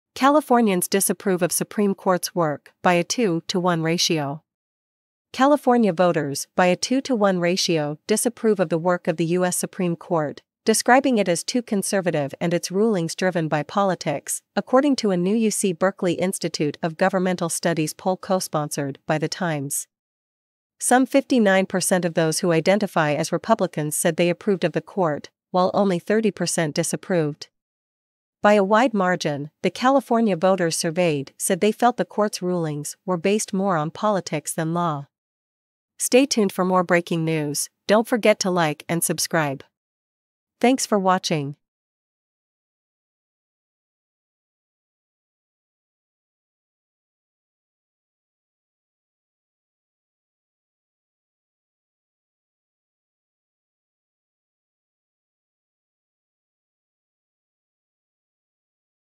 Californians disapprove of Supreme Court's work by a 2-1 ratio
California voters by a 2-1 ratio disapprove of the work of the U.S. Supreme Court, describing it as too conservative and its rulings driven by politics, according to a new UC Berkeley Institute of Governmental Studies poll co-sponsored by The Times. Some 59% of those who identify as Republicans said they approved of the court, while only 30% disapproved.  By a wide margin, the California voters surveyed said they felt the court’s rulings were based more on politics than law.